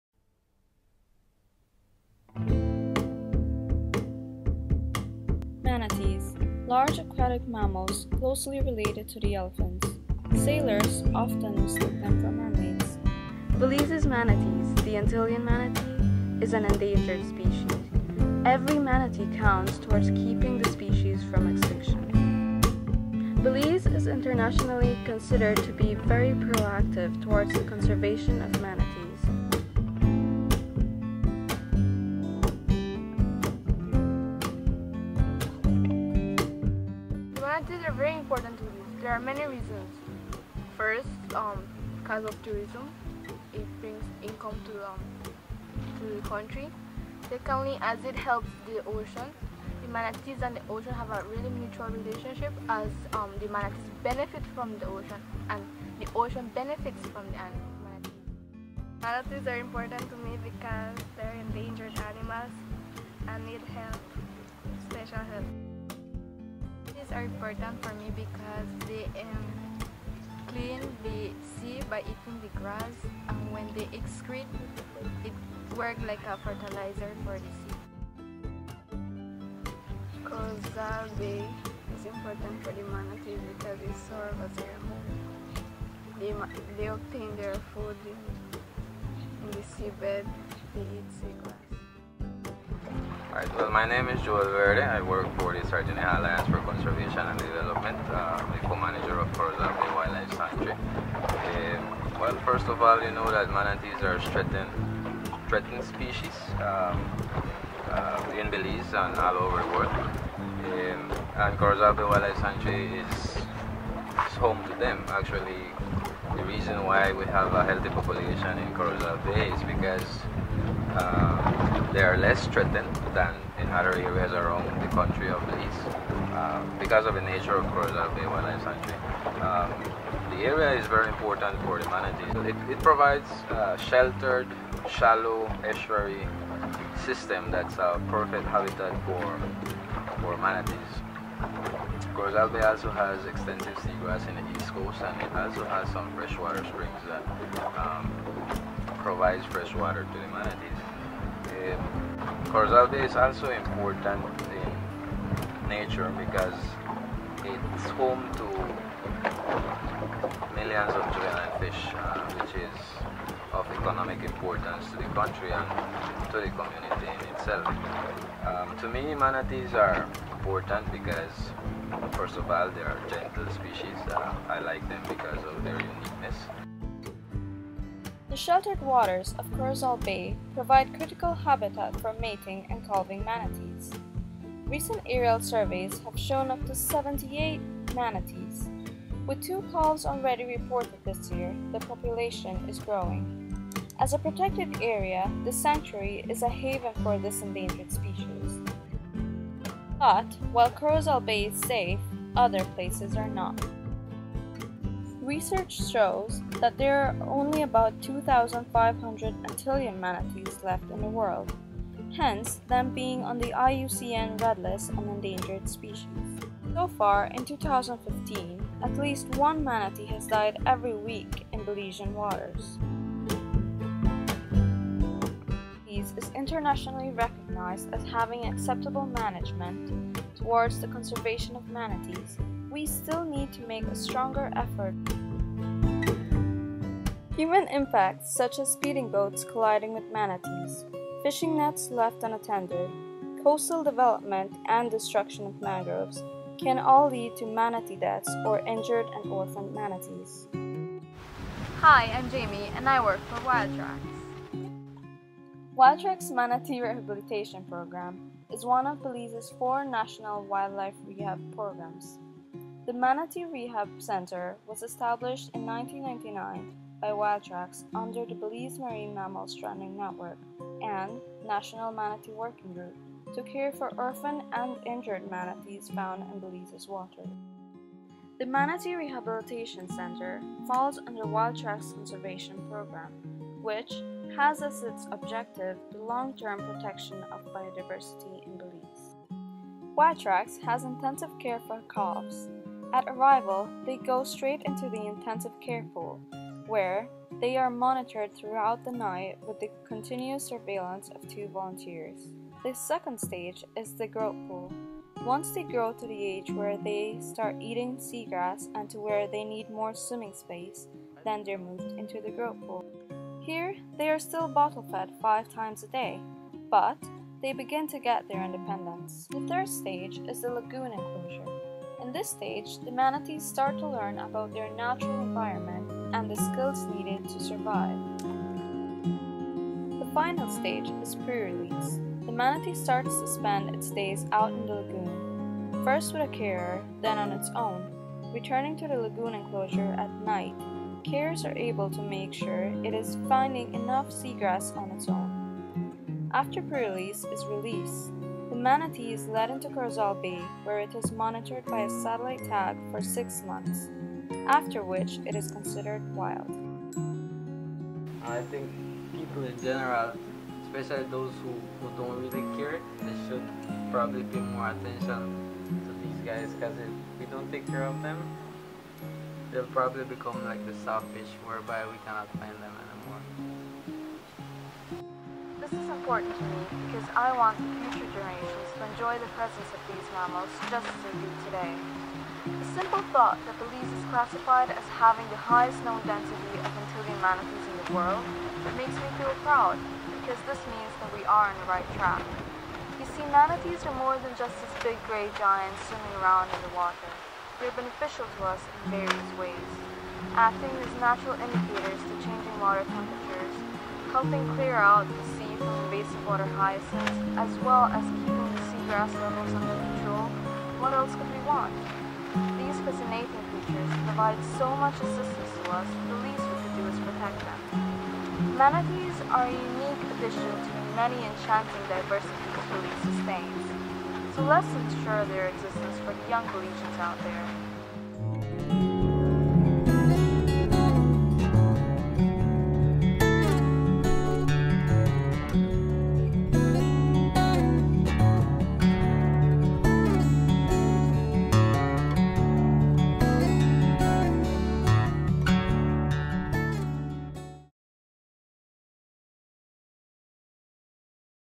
Reef Week 2015 Manatee Conservation in Belize
Manatees - Why are they important? This Reef Week 2015, Wildtracks  and its partners at SACD have put together a short video to explore manatees, and why they are important to Sarteneja, and to Belize. Manatees in the wild, manatees in rehabilitation... Enjoy!